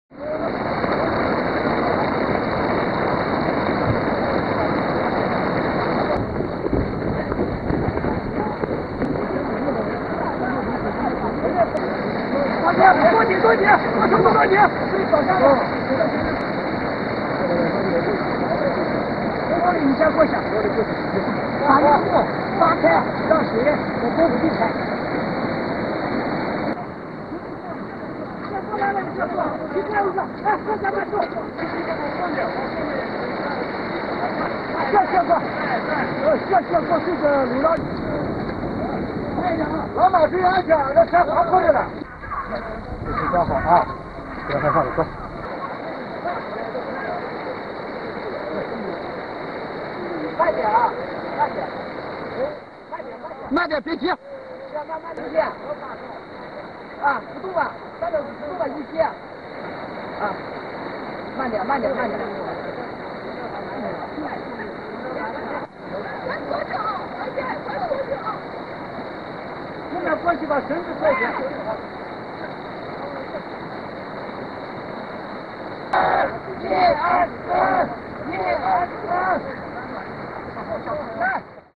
73 villagers rescued from torrential flooding in China 07 26 2013
A deluge of rain hit Tianshui in northwest Gansu province in China, destroying more than 80 houses and isolating 73 Duijawan villagers by cutting off roads to the village. Soldiers created makeshift bridges from tree trunks to evacuate the community, which included a two-month-old baby.